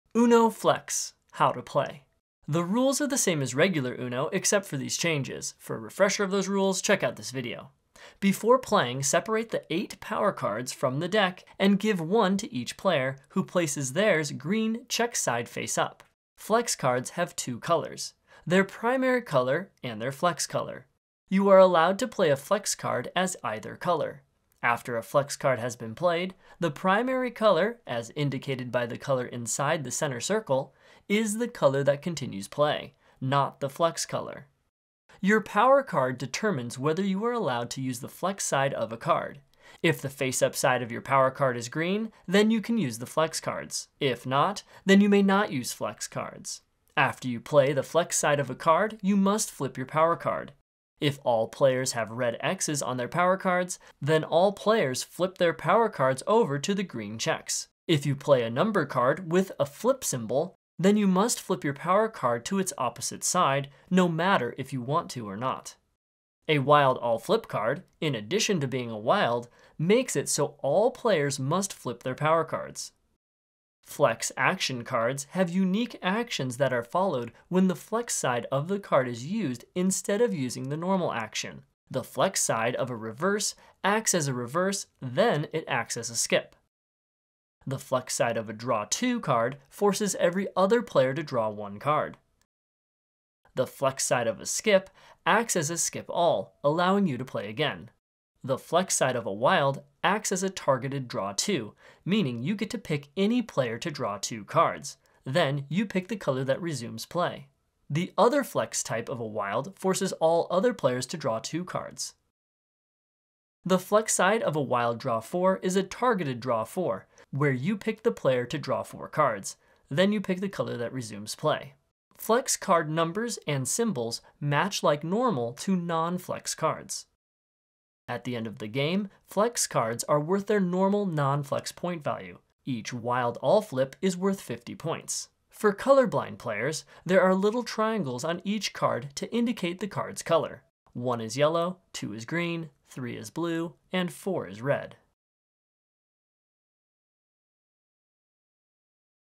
How to play Uno Flex
Learn the rules to the card game Uno Flex quickly and concisely - This video has no distractions, just the rules.  For a refresher of the original rules of Uno, check out this video

RULES:
The rules are the same as regular uno except for these changes.  Before playing, separate the 8 power cards from the deck and give 1 to each player, who places theirs green check-side faceup. Flex cards have 2 colors. Their primary color and their flex color. You are allowed to play a flex card as either color. After a flex card has been played, the primary color, as indicated by the color inside the center circle, is the color that continues play, not the flex color.

Your power card determines whether you are allowed to use the flex side of a card. If your power card is the green check face up, then you can use flex cards; if not, then you may not use flex cards. After you play the flex side of a card, you must flip your power card. If all players have red x’s on their power cards, then all players flip their power cards to the green checks. If you play a number card with a flip symbol, then you must flip your power card to its opposite side, no matter if you want to or not.

A wild all flip card, in addition to being a wild, makes it so all players must flip their power cards. Flex action cards have unique actions that are followed when the flex side of the card is used instead of using the normal action. The flex side of a reverse acts as a reverse then it acts as a skip. The flex side of a draw 2 card forces every other player to draw 1 card. The flex side of a skip acts as a skip all, allowing you to play again. The flex side of a wild acts as a targeted draw 2, meaning you get to pick any player to draw 2 cards, then you pick the color that resumes play. The other flex type of a wild forces all other players to draw 2 cards. The flex side of a wild draw four is a targeted draw 4, where you pick any player to draw 4 cards, then you pick the color that resumes play.

At the end of the game flex cards are worth their normal non-flex point value. Each Wild all flip is worth 50 points. For colorblind players, there are little triangles on each card to indicate the card’s color. 1 is yellow, 2 is green, 3 is blue, and 4 is red.